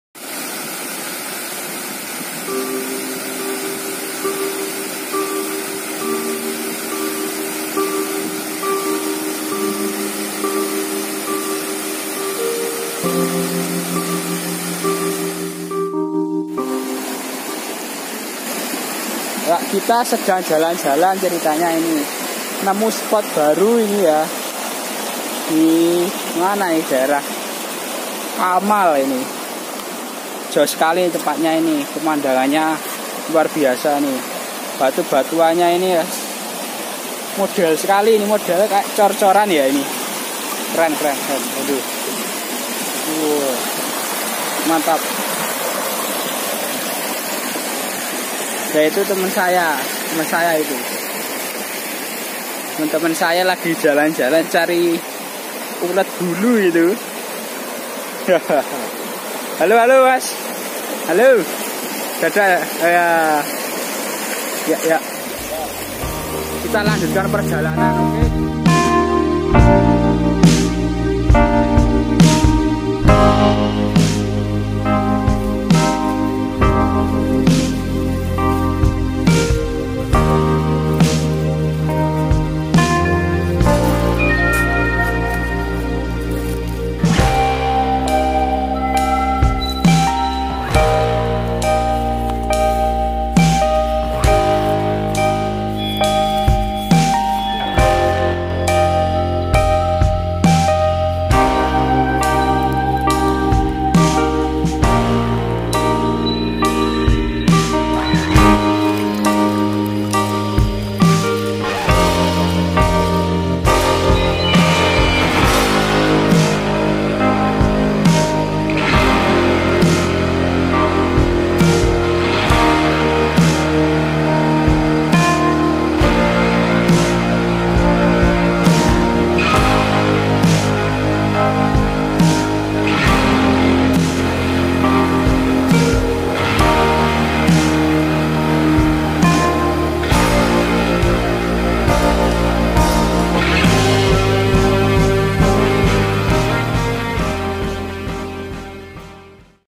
Wisata deket WADUK SERMO!!!
Video yg semoga saja menghibur dan bisa dinikmati 👍

Ceritanya sih ya, jalan-jalan dari SERMO terus blasakan sampe akhirnya kesasar :"